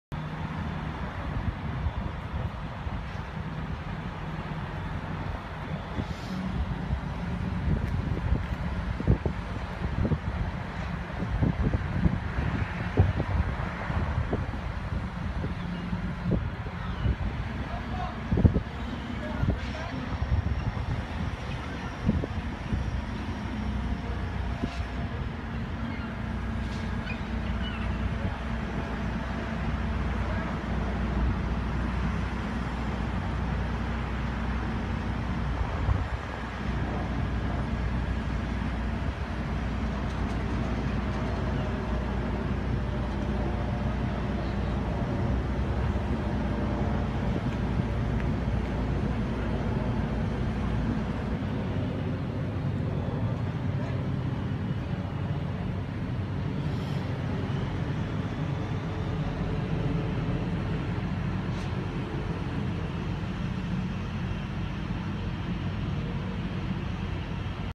Modular unit breaks loose
A section of a modular unit fell onto Pennsylvania Avenue